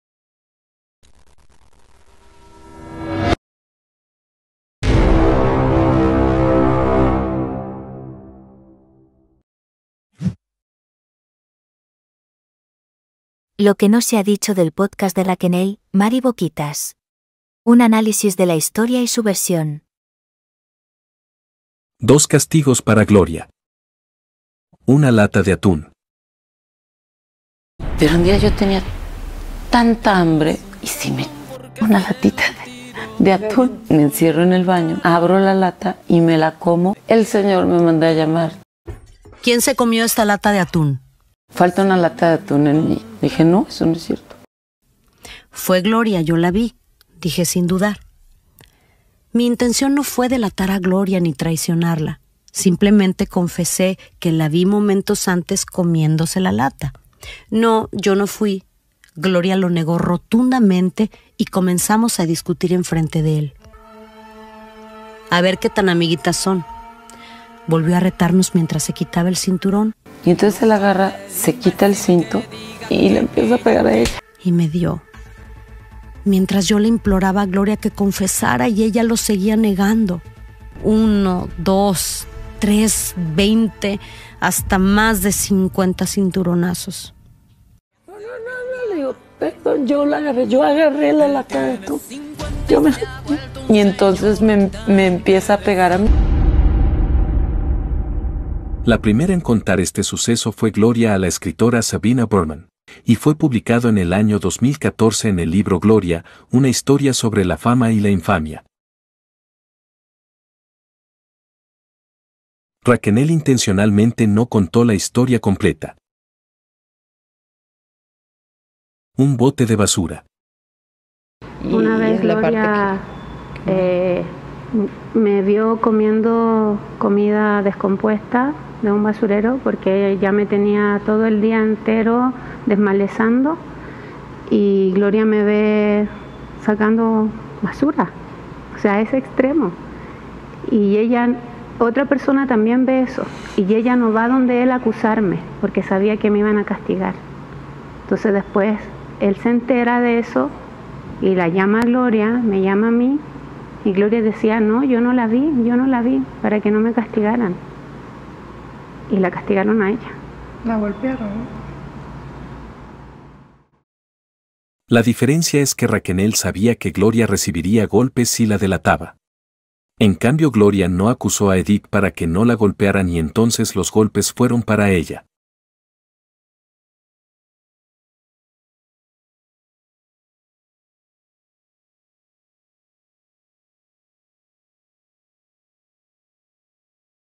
Lo que NO se ha dicho del podcast de Raquenel (Mary Boquitas) | Dos castigos para Gloria Trevi
LO QUE NO SE HA DICHO DEL PODCAST DE RAQUENEL (Mary Boquitas)
Analizaremos la versión de Raquenel sobre los comentarios que hace de Gloria en su podcast y demostraremos con pruebas sus mentiras y omisiones.

1ra. Parte. “Dos castigos para Gloria”